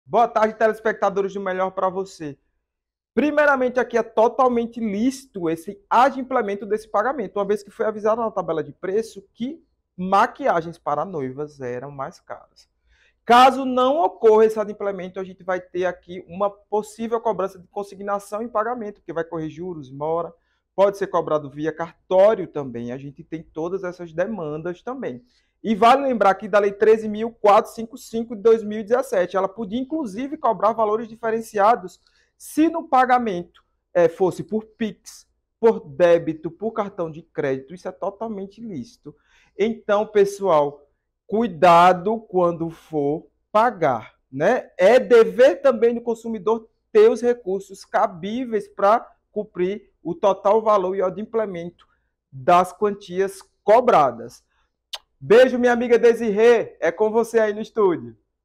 O QUE DIZ A LEI? Maquiadores podem cobrar mais caro por maquiagem de noiva?
Você acompanhou a polêmica da maquiagem de noiva que rolou na internet?

Uma maquiadora fez uma maquiagem social em uma noiva. Quando ela soube do fato, a profissional se sentiu enganada.

Mas o que a lei diz sobre isso? O advogado Jurandir Pereira fala pra gente!